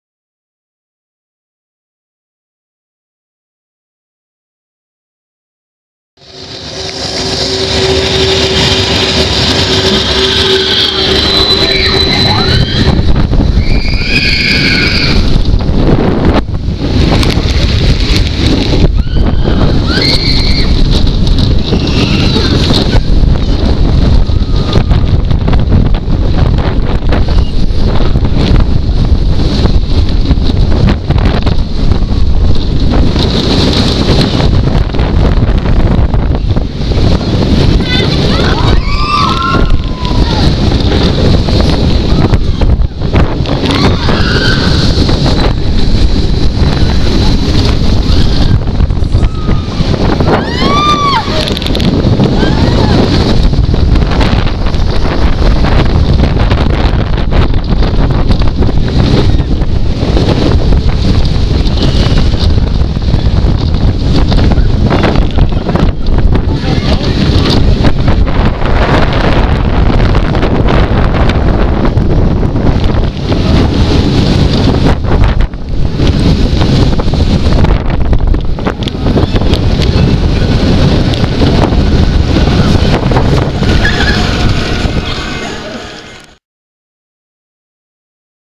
ICON - Blackpool Pleasure Beach - POV - 5|6|2024
A 2024 Front Seat POV of ICON at Blackpool Pleasure Beach! Enjoy!